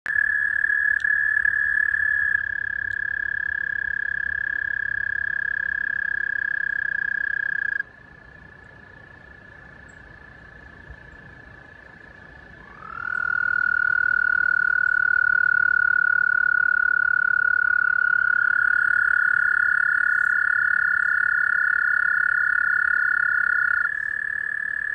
Amazing Vibrations in Water from Toad Chirps || ViralHog
Occurred on April 6, 2020 / Newark, Delaware, USA

"Lauren was hiking in White Clay Creek and came across a side stream where lots of toads were chirping. She went to film the sound from the toad and just happened to capture it at the right time. The toads call in the water is a perfect example of cymatics in nature. Vibration influences matter."